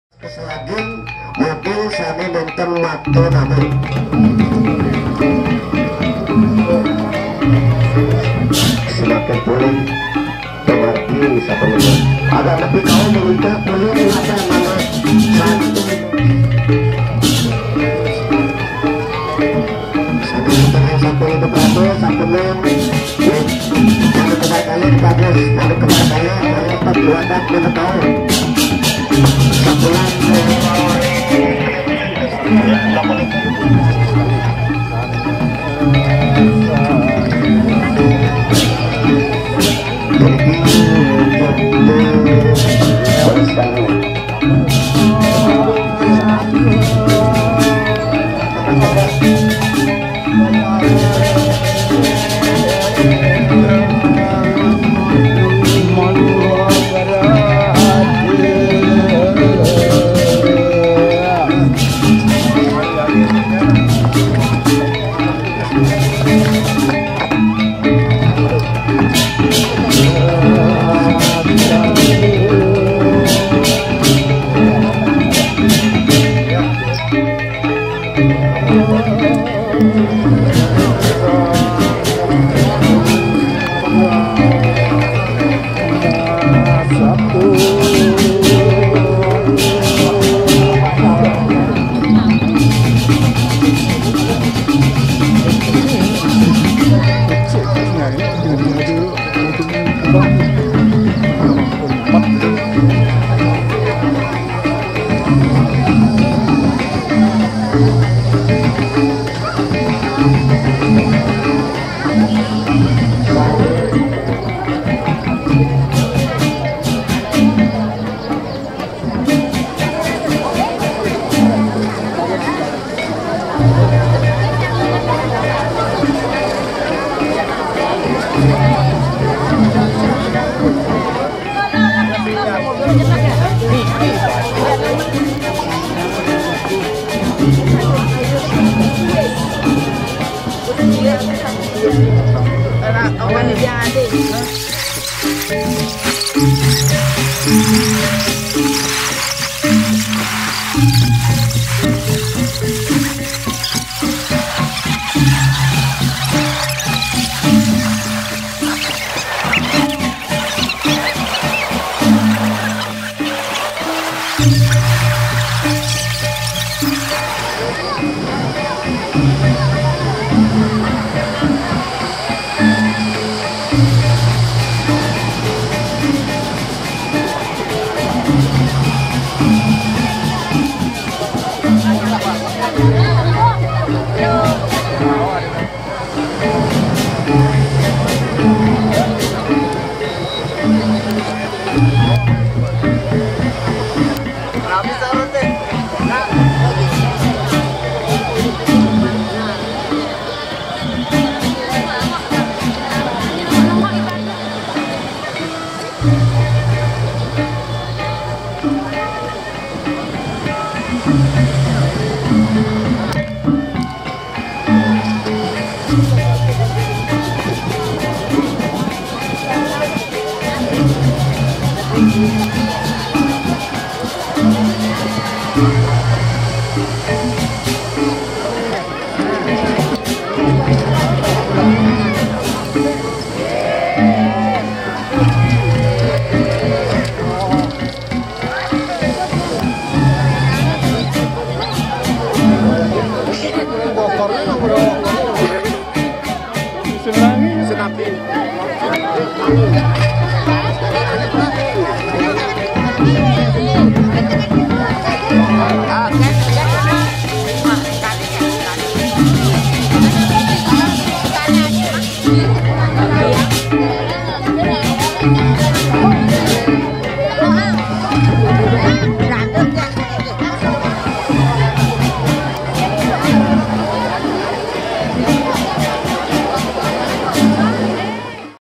NGASTI SOMA KLIWON UYE 13 8 2018 KARYA ATMA WEDANA BALIGIA JUNGUTAN
Ngasti
Ngasti adalah proses menghanyutkan hasil peleburan yang berupa abu ke laut dengan seluruh peralatan seperti Padma, Bukur, Naga, Empas, Angsa, dan peranghat lainnya atau yang disebut juga dengan artawingka (sesuatu hal yang sudah rusak tetapi masih dimuliakan)